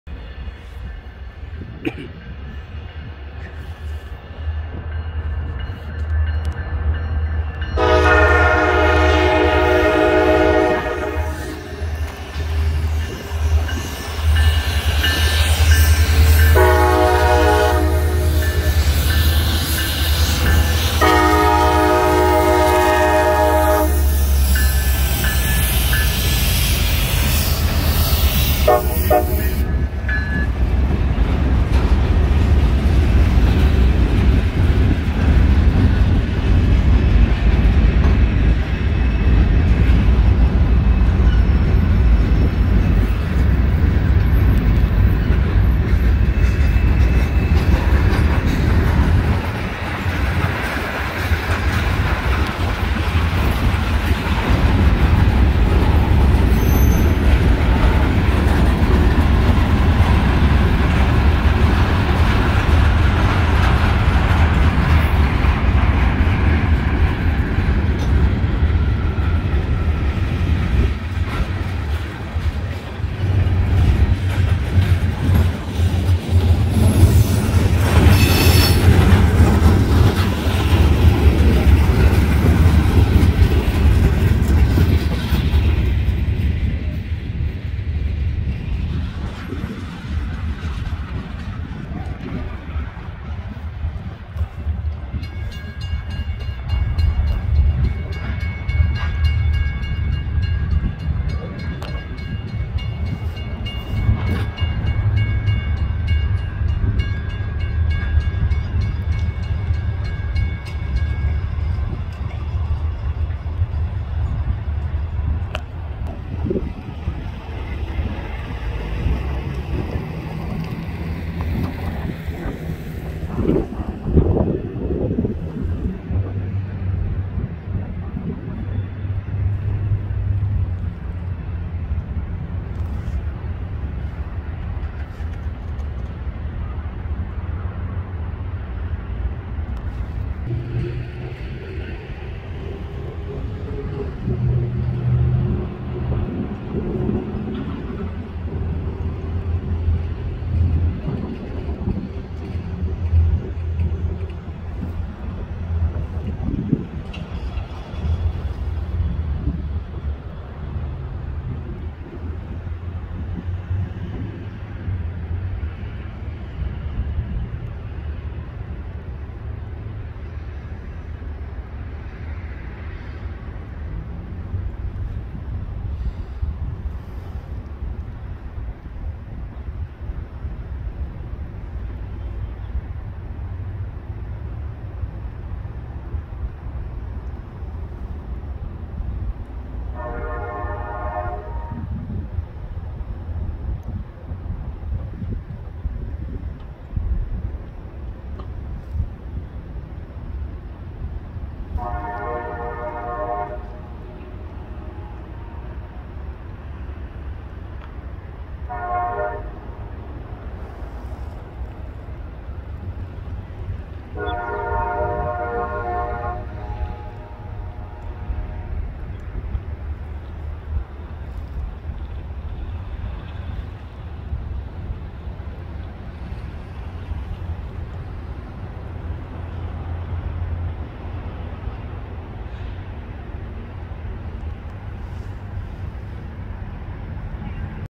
Csx C706 15 Cars prt. 1
Location.
Cc Subdivision, Winchester, Ky, Main Street.

Date Taken 01/24/22 2:43 PM

Locomotive Power.
Csx Locomotive No. 2016
Csx Locomotive No. 6048